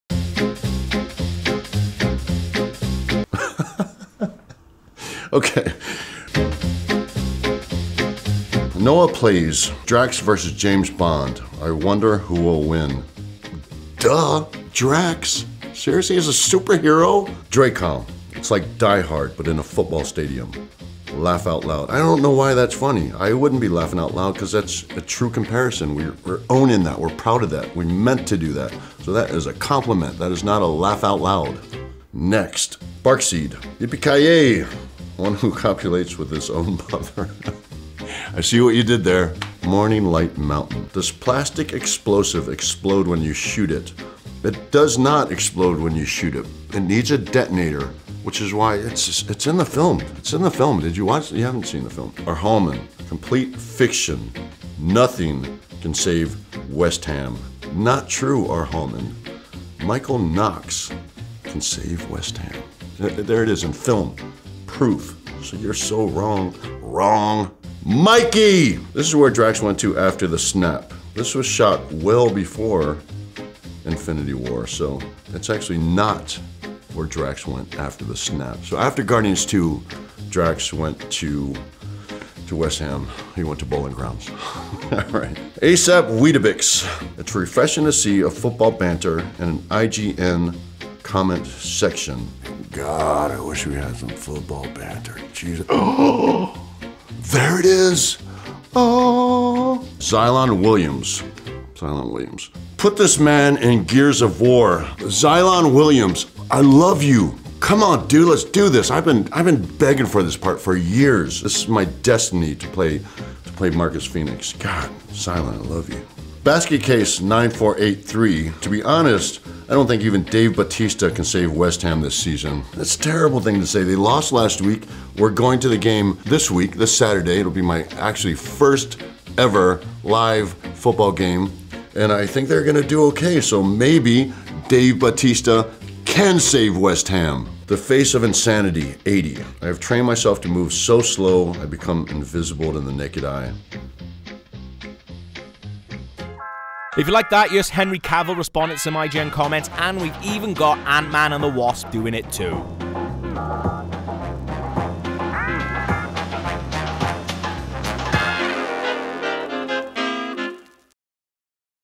Dave Bautista Responds to IGN Comments
IGN has one of the most active comments sections on the Internet. We showed Dave Bautista some of our favourites left for the Final Score trailer.

Follow IGN for more!